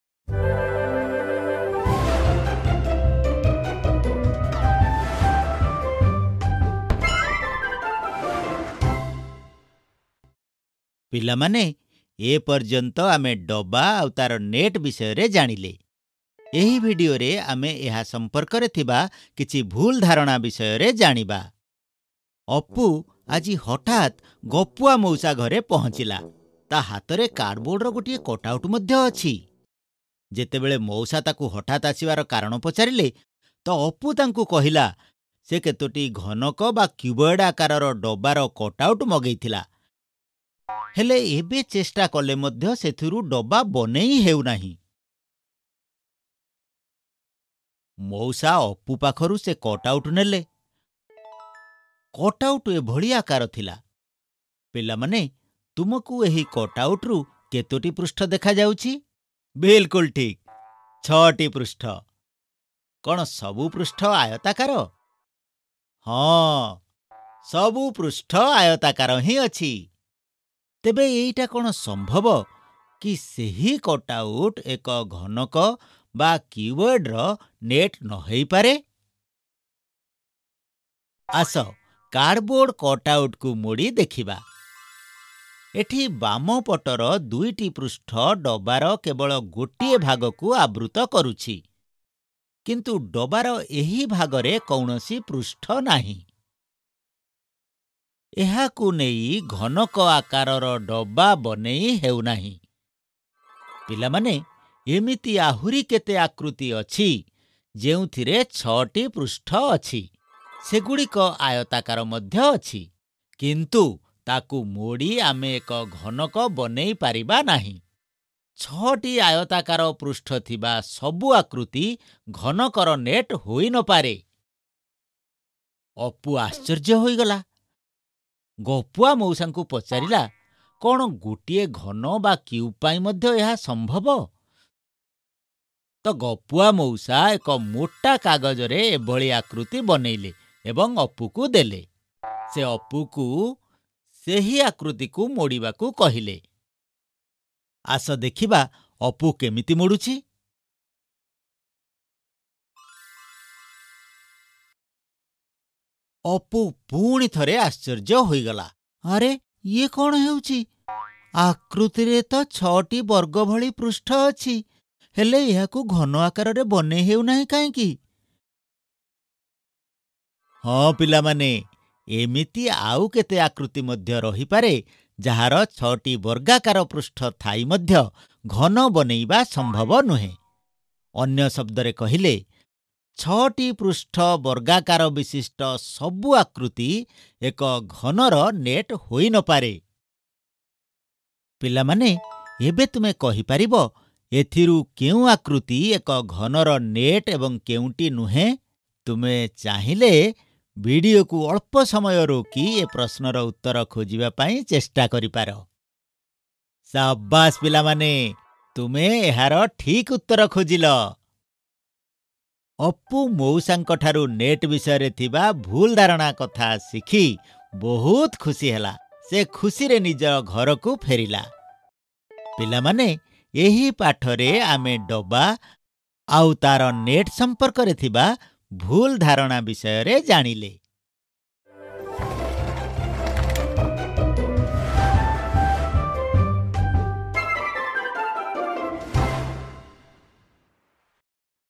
ବାକ୍ସ ଏବଂ ତାହାର ନେଟ - ଘନକ | Part 4/4 | Boxes and their net: Cuboid | Odia | Class 5
ବାକ୍ସ ଏବଂ ତାହାର ନେଟ - ଘନକ | Part 4/4 | Boxes and their net: Cuboid | Odia | Class 5 | ଆକୃତି ଓ ସ୍ଥାନ  | कक्षा 5 | NCERT | ବାକ୍ସ ଓ ରେଖାଚିତ୍ର

Boxes and their net: Cuboid | Shapes and Space | Grade 5 | NCERT | Boxes and Sketches

Welcome to part 4 of Boxes and their net: Cuboid. In this video, we will discuss various misconceptions and/or common errors related to Boxes and their net: Cuboid.